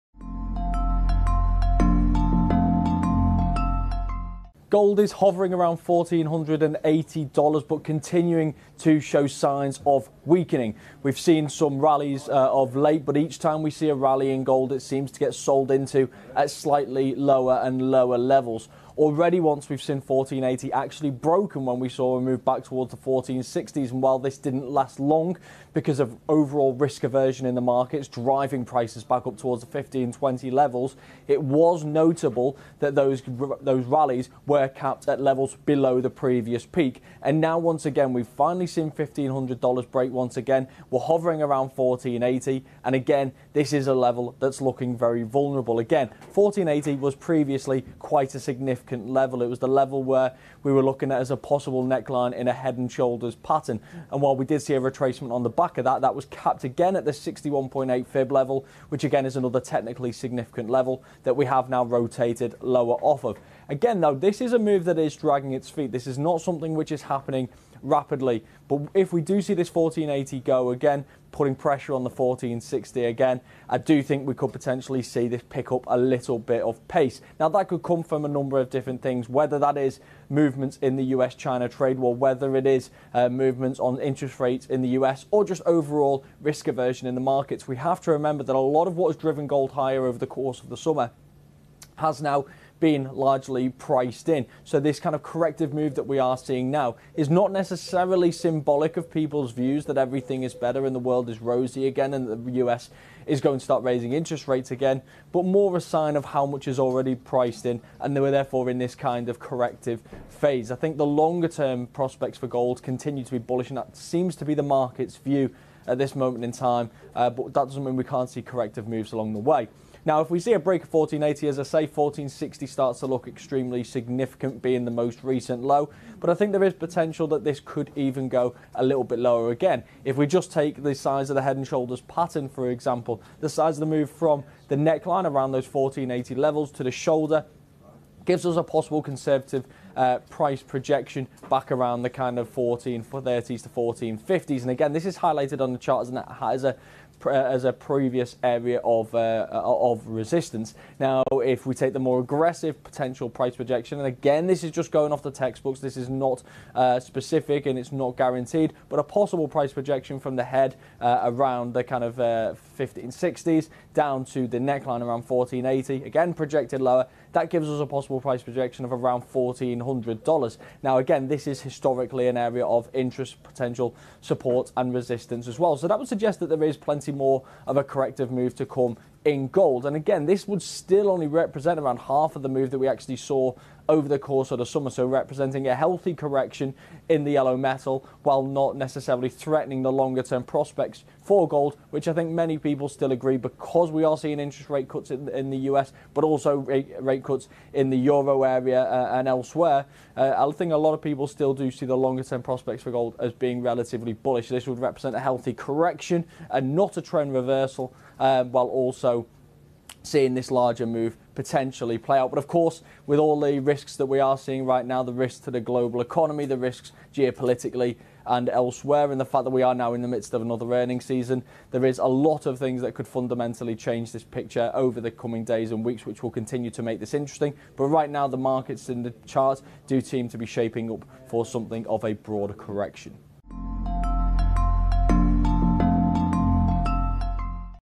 Gold Looking Vulnerable
Craig Erlam, Senior Market Analyst at OANDA takes a look at the yellow metal.